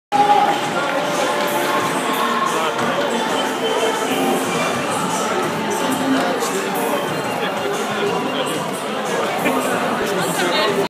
Проводы Уфы
Болельщики провожают любимую команду на зимний перерыв, хотя впереди еще матч с Томью